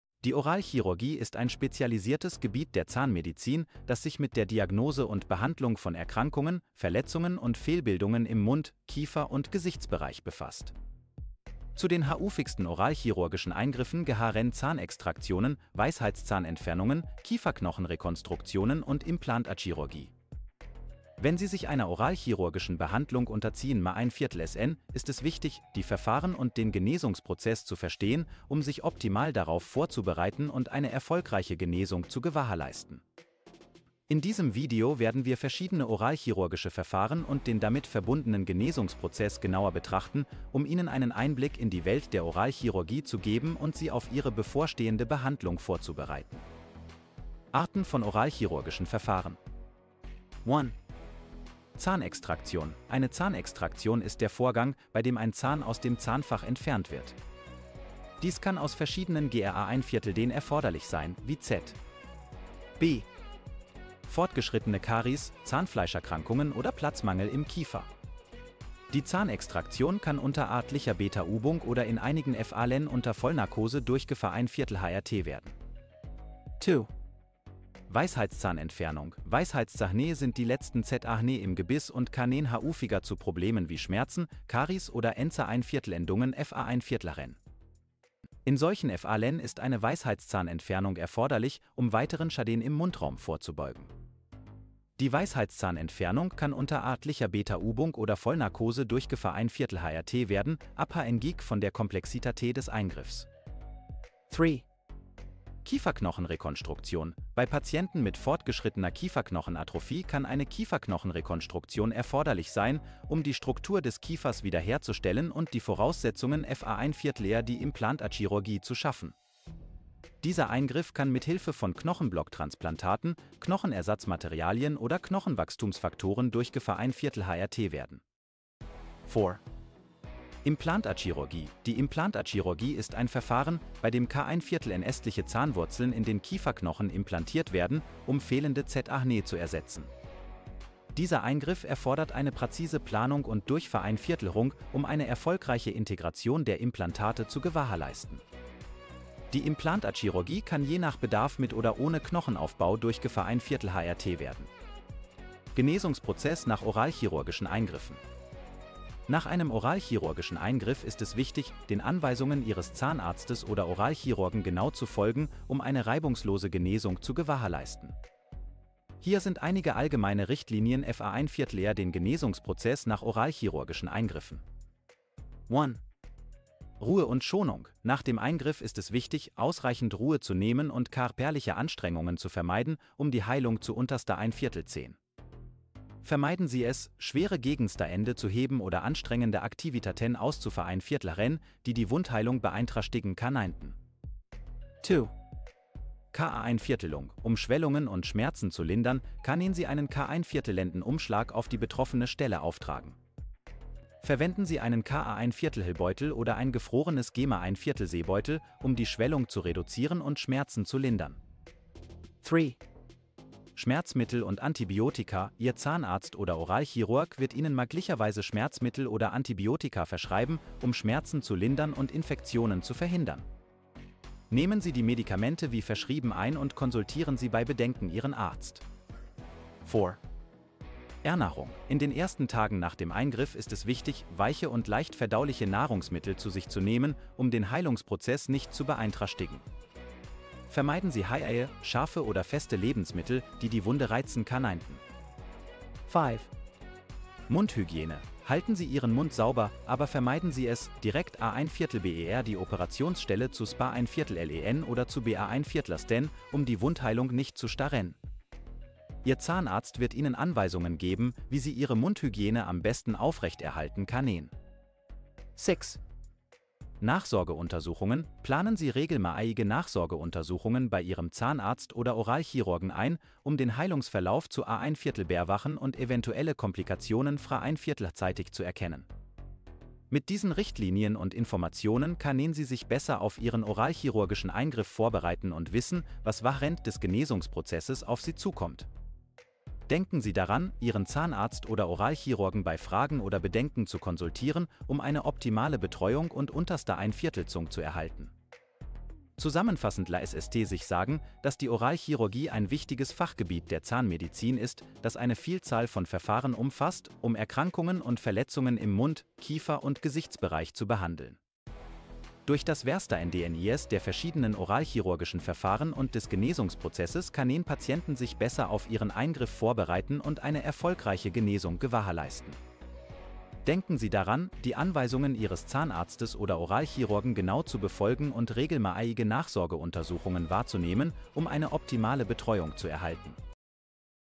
Oralchirurgie: Was sind die häufigsten Eingriffe und wie verlaufen sie? - Erfahren Sie alles Wicht
Kontaktiere uns:
Telegram Die Oralchirurgie umfasst die Behandlung von Erkrankungen, Verletzungen und Fehlbildungen im Mund-, Kiefer- und Gesichtsbereich. Dazu gehören unter anderem Zahnextraktionen, Wurzelspitzenresektionen, die Entfernung von Weisheitszähnen, die Korrektur von Kieferfehlstellungen und die Versorgung mit Zahnimplantaten. Auch die Diagnose und Behandlung von Kiefergelenksproblemen sowie die Durchführung von Kiefer- und Gesichtsoperationen zählen zu den Aufgaben der Oralchirurgie. Das Ziel ist es, die Funktion und Ästhetik des Mund- und Kieferbereichs wiederherzustellen und die Gesundheit der Patienten zu verbessern.